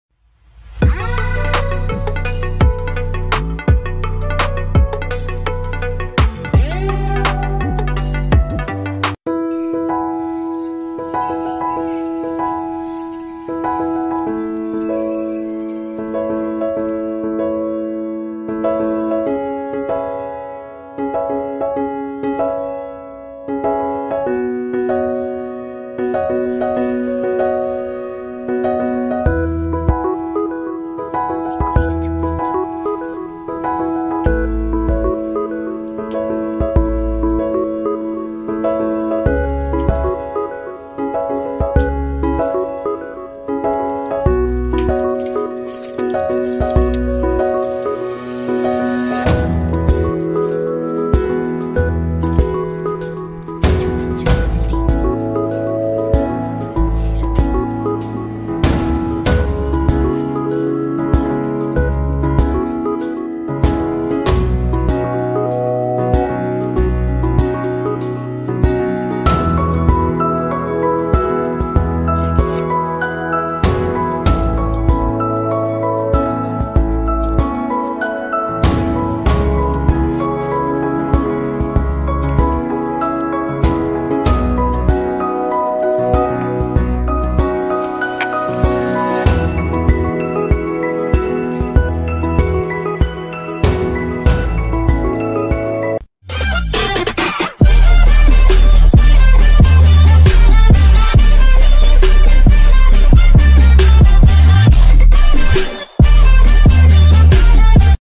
my success in harvesting plants
Hi guys my name is ammu and welcome to my channel and in this video I am gonna show my success in plants and harvesting and please subscribe like and share